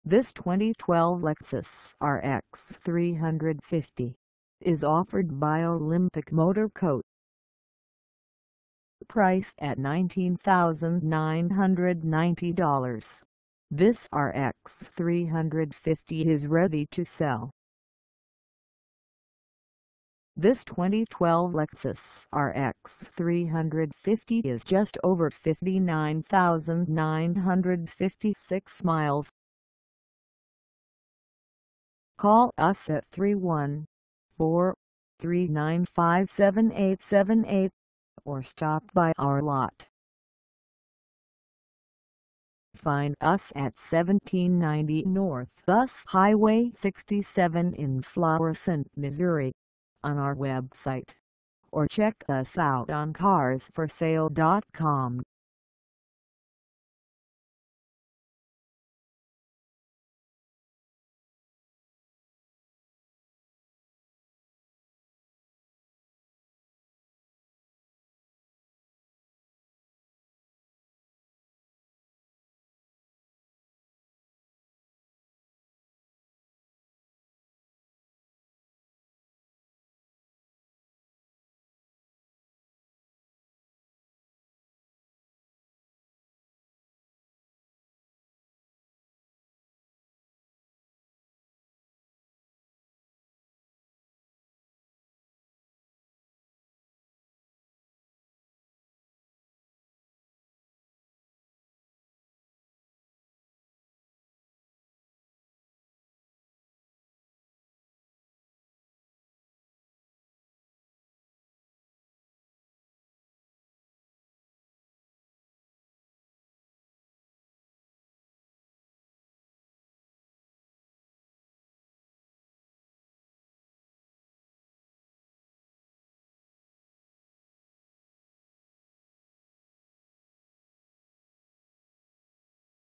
2012 Lexus RX 350 Base 4dr SUV for sale in Florissant, MO 63
This 2012 Lexus RX 350 Base 4dr SUV is for sale in Florissant, MO 63033 at OLYMPIC MOTOR CO.

Engine: 3.5L V6
Transmission: Automatic 6-Speed
Color: Nebula Gray Pearl
Mileage: 59956
Contact OLYMPIC MOTOR CO 1790 N US Highway 67 Florissant, MO 63033 to test drive this 2012 Lexus RX 350 Base 4dr SUV today.